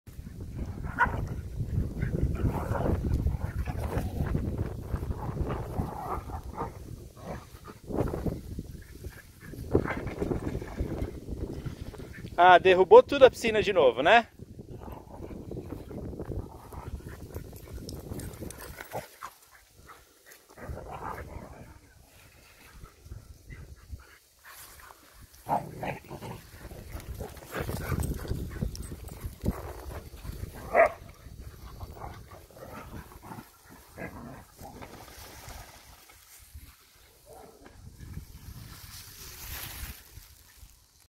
Gramado para border collie testado com sucesso...
Ah, que delícia,  logo logo teremos um espaço para correr loucamente sem parar! Típico dos border collies...
E olha como a Luna está bem! Mais de 11 anos e correndo pacas!